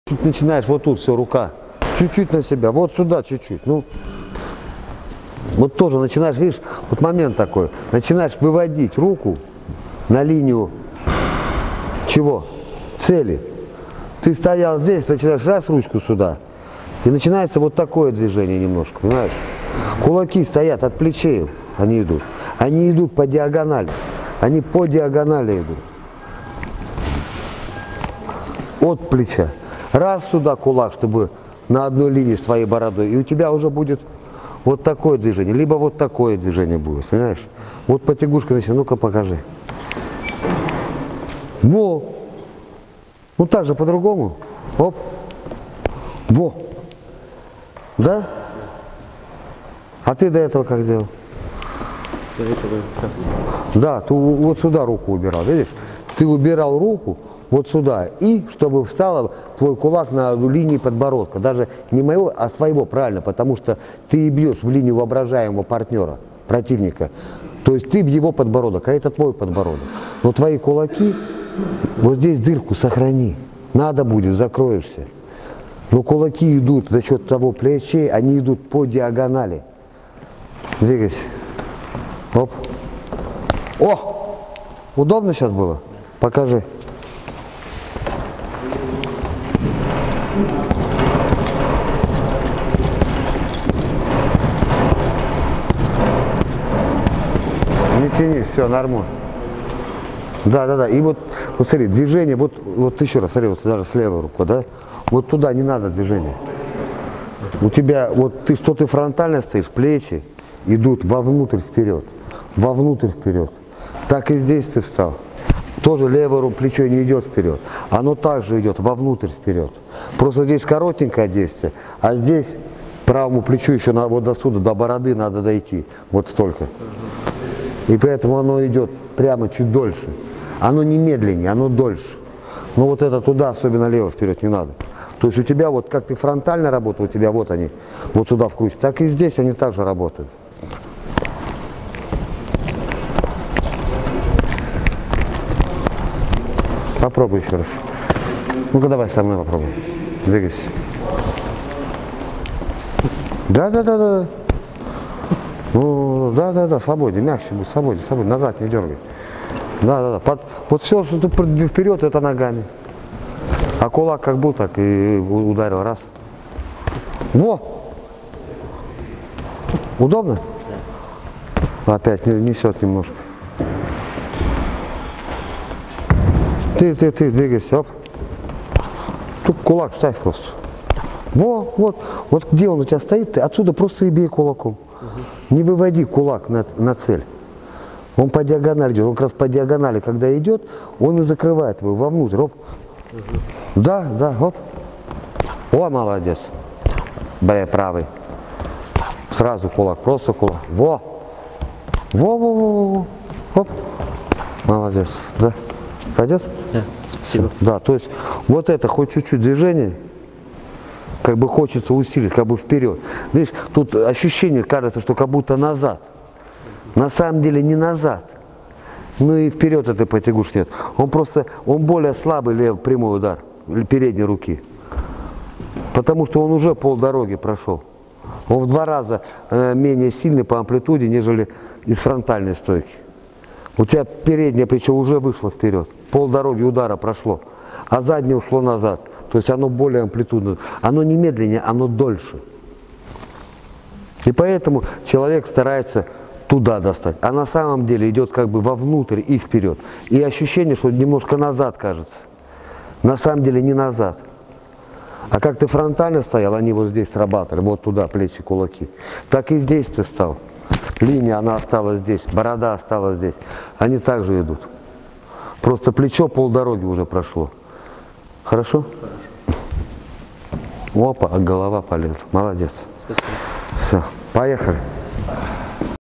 Бокс: пред-ударное положение кулака (English subs)
Правильное положение кулака перед прямым ударом. Избегать "потягушек". Записывайтесь на занятия! т. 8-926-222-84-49. Где проходят тренировки О тренере Стоимость занятий